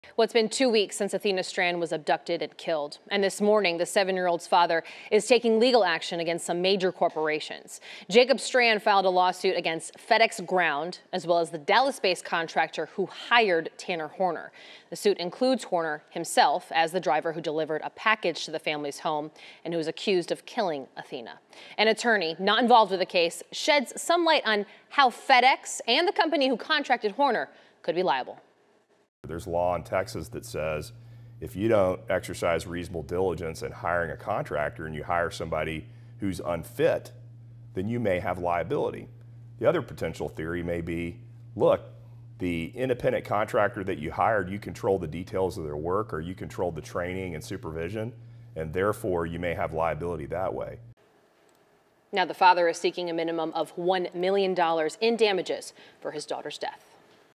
Athena Strand's father suing FedEx, murder suspect, contractor that hired him
The lawsuit was filed Tuesday in Wise County District Court. Strand's father is seeking over $1,000,000 in damages and a trial by jury, according to the lawsuit.